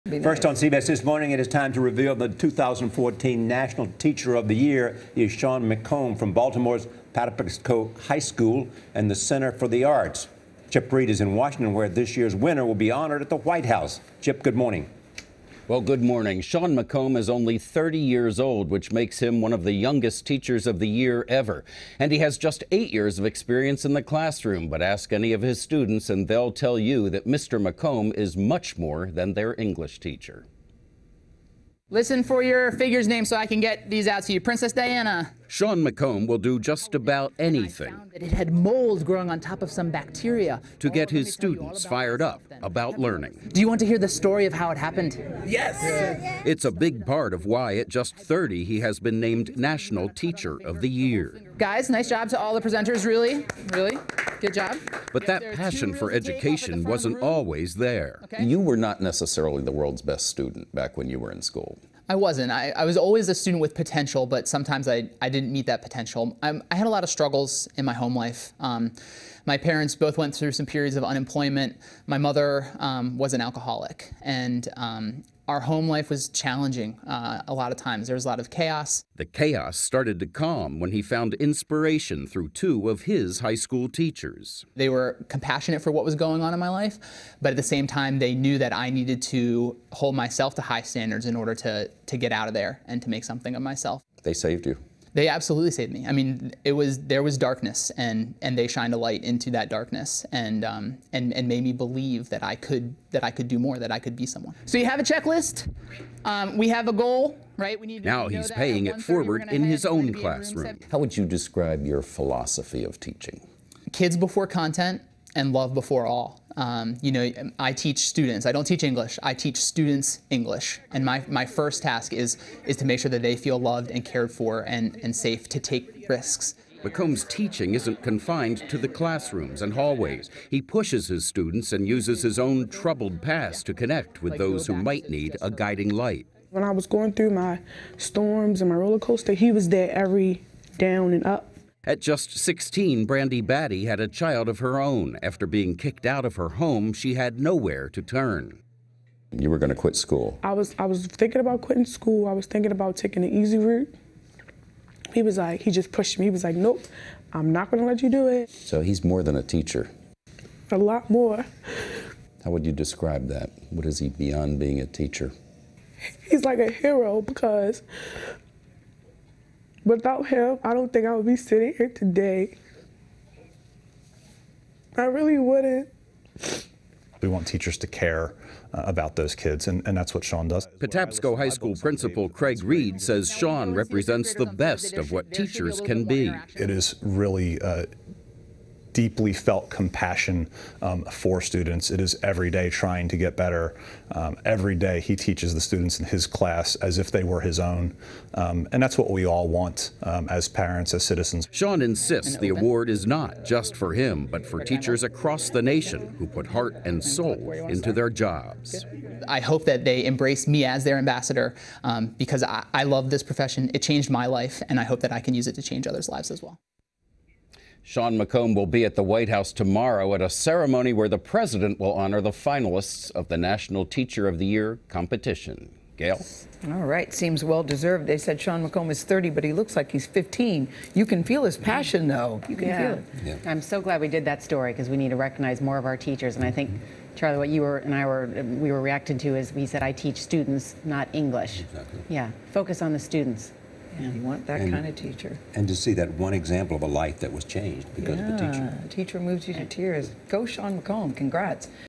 Teacher of the Year: Baltimore's Sean McComb given top honor
The 2014 National Teacher of the Year award will be honored at the White House. Chip Reid reports on this year's winner, Sean McComb, who is one of the youngest recipients ever.